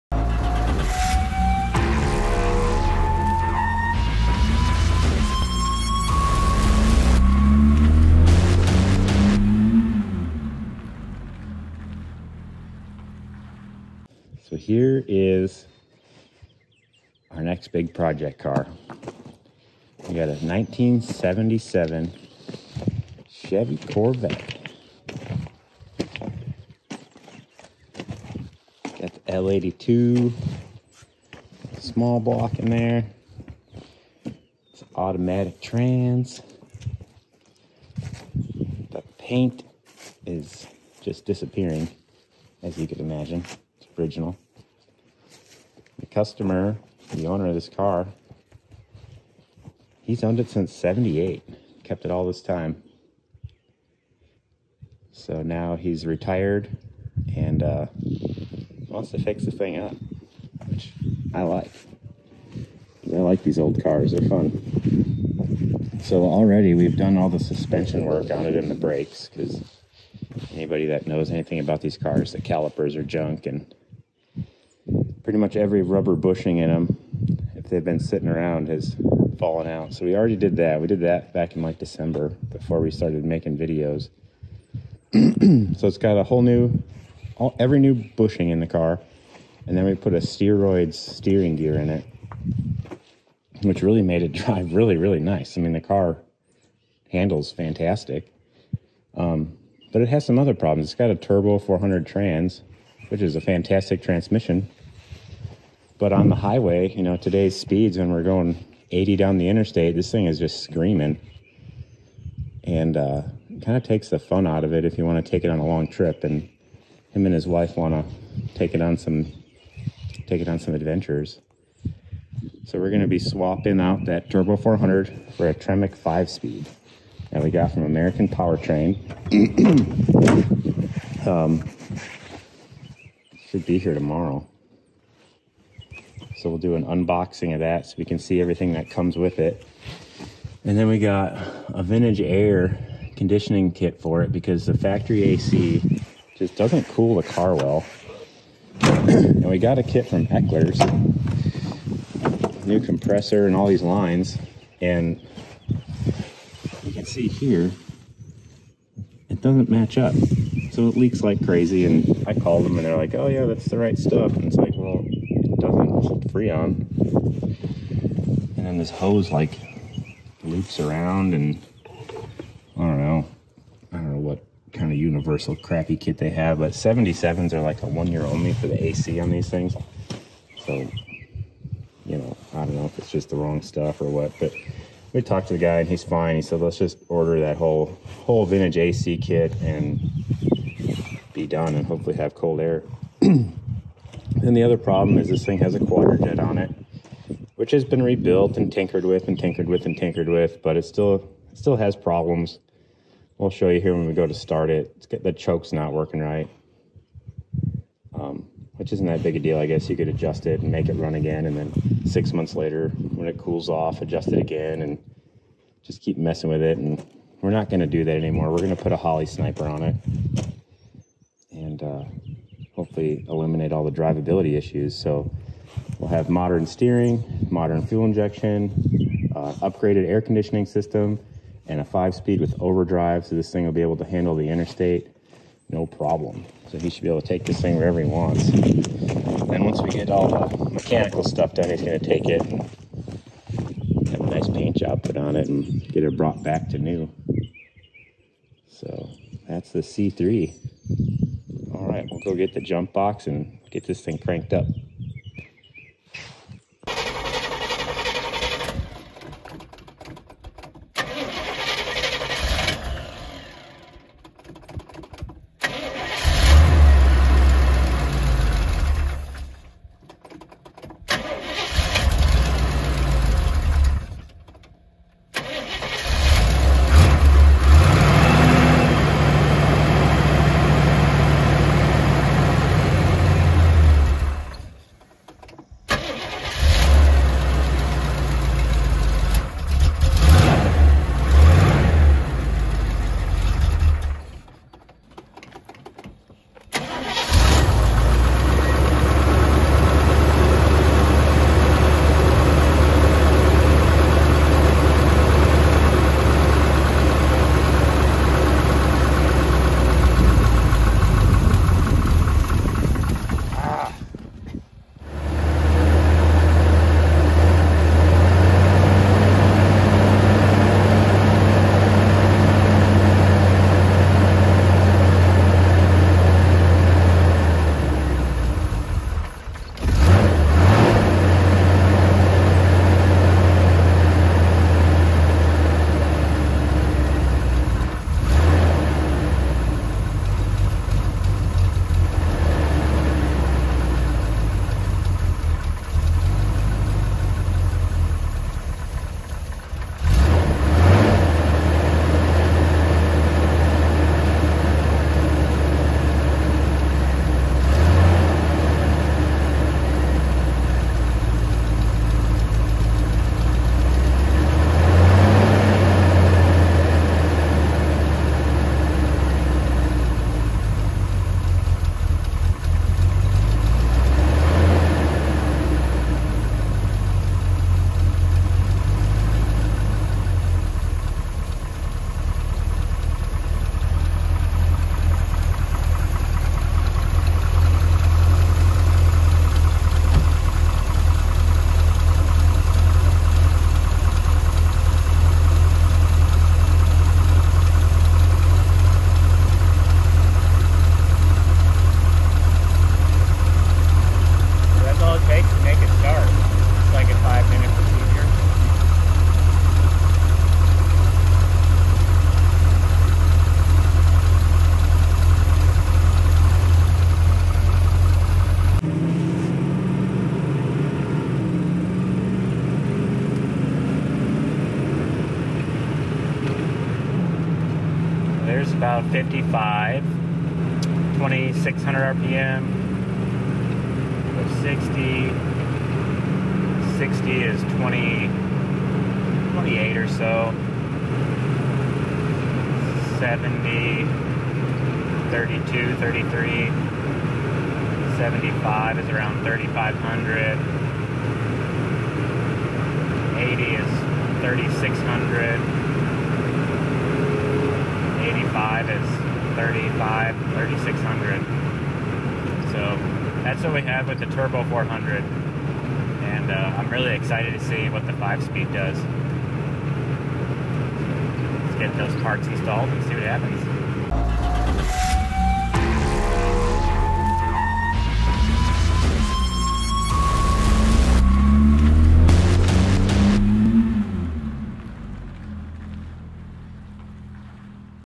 Major Upgrades For This 77 Corvette
This 77 Corvette will be updated with a new Tremec TKX 5-Speed Transmission using a kit from American PowerTrain, a new Vintage Air conditioning system and a new Holley Sniper fuel injection system.  Follow us as we tear into this Corvette and install these upgrades so the owner can take it back out on the open road for some fun adventures.